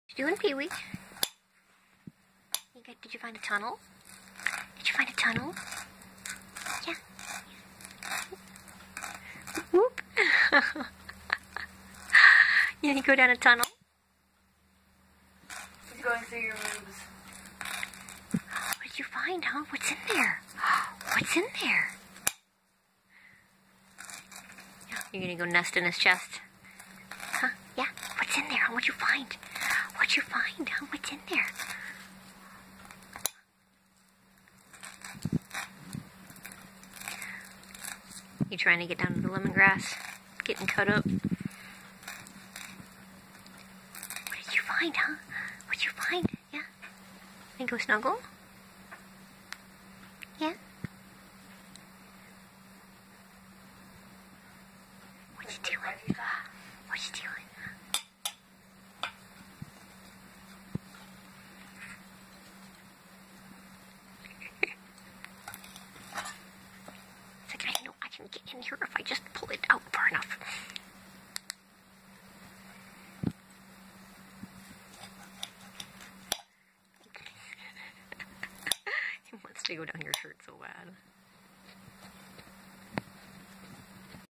Funny bird is obsessed with chests! Parrot Conure AFV animal
My rescue bird Piwi has a strange obsession with tunneling down people's chests haha. Here he is attacking my sweetie's chest while he chopped Lemongrass for the soup we were making. He got so excited trying to stick his face down his shirt!